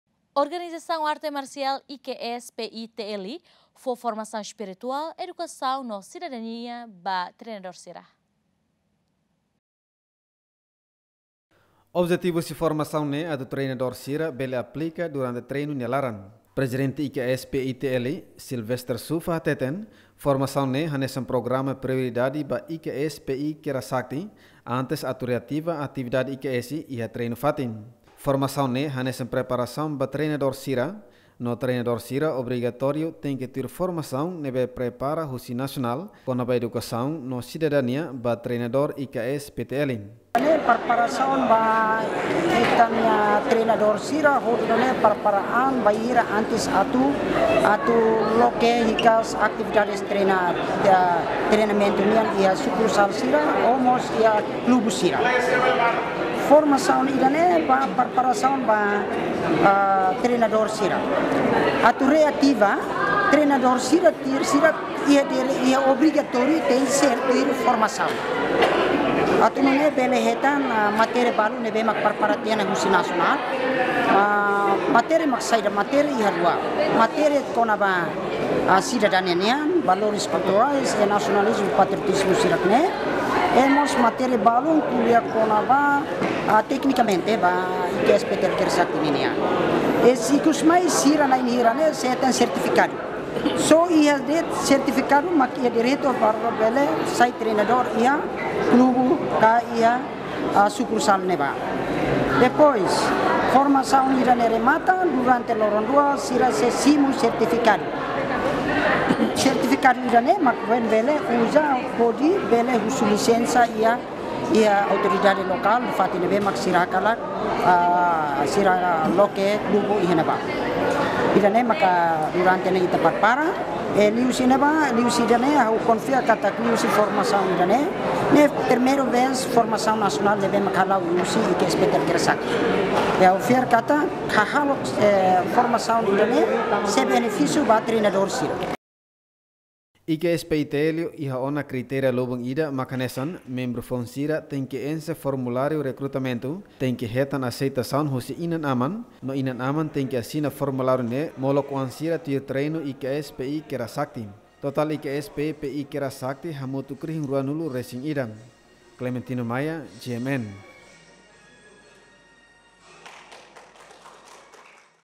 (IKS PI TL) fo formasaun espirituál, edukasaun no sidadania ba treinador sira
Organizasaun Arte Marsiál IKS PI TL fo formasaun espirituál, edukasaun no sidadania ba treinador sira